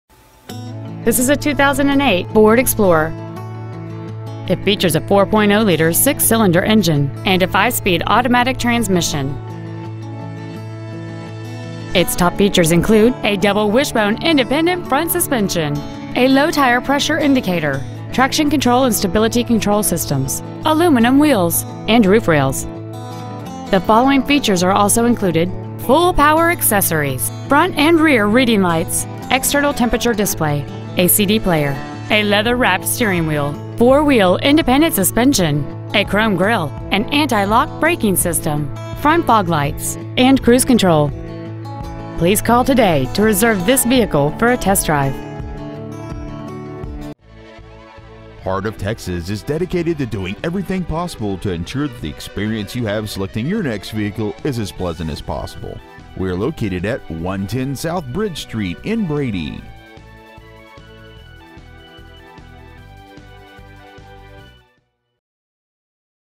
2008 Ford Explorer Brady TX
We have been honored to serve the Brady TX area , we promise that your experience at our dealership will exceed your expectations !

 Year : 2008
 Make : Ford
 Model : Explorer
 Engine : 4.0L V6 12V MPFI SOHC
 Trans . : 5-SPEED AUTOMATIC
 Exterior : WHITE
 Miles : 80,086

 Stock : 879005

 Heart of Texas Ford, Inc.